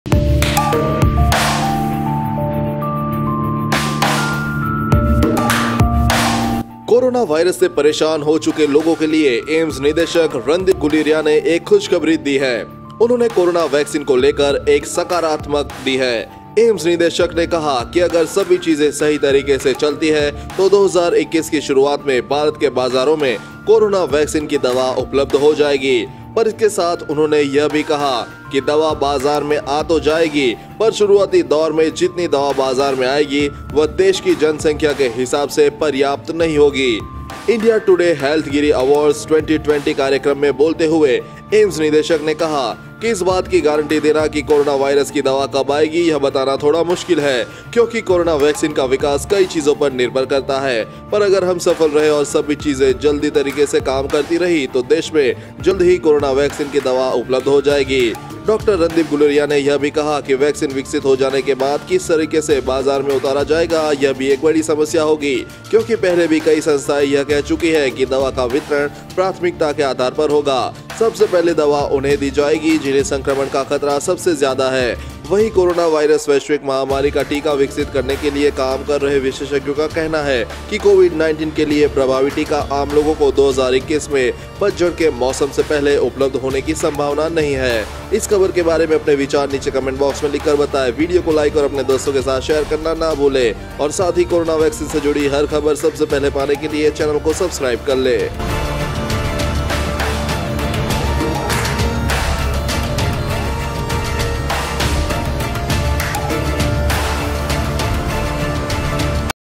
Covid-19 Vaccine: AIIMS के निदेशक Randeep Guleria ने बताया कब तक आयेगी Corona Vaccine !
Coronavirus Vaccine: Aiims Chief Randeep Guleria Said Vaccine May Come By January 2021 But Raised This Concern | Corona Vaccine Latest News & Updates

EDITOR ►Manjeet